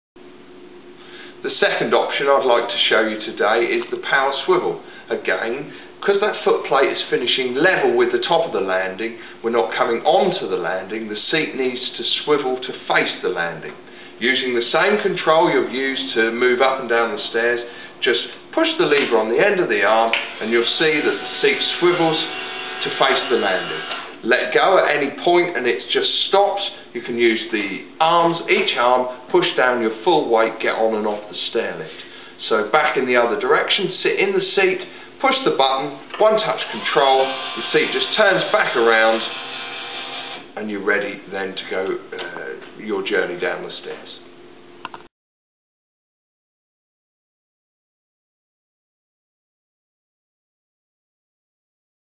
Stairlift Powered Swivel Seat
Description of a powered swivel seat on a stairlift by Norfolk Stairlifts